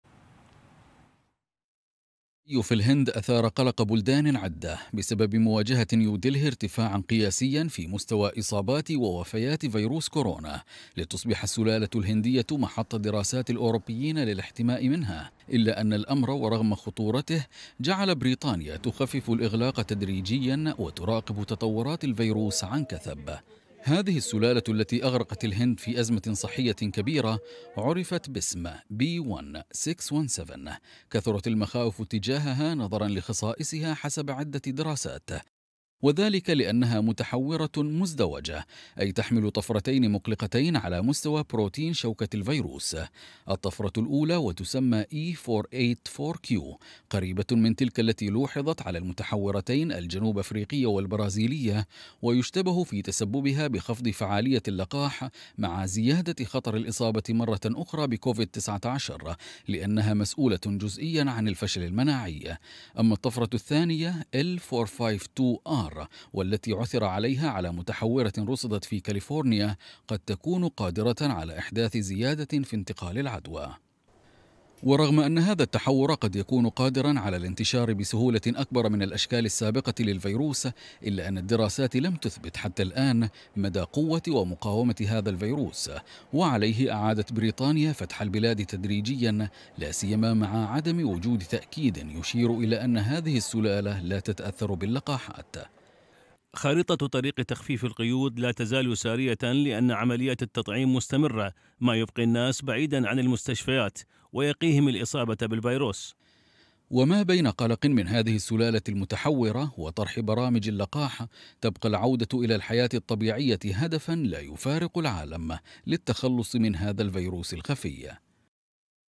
ما خصائص كورونا الهندي الذي أرعب العالم؟
متحور مزدوج وينتشر بسهولة.. ما خصائص كورونا الهندي الذي أرعب العالم؟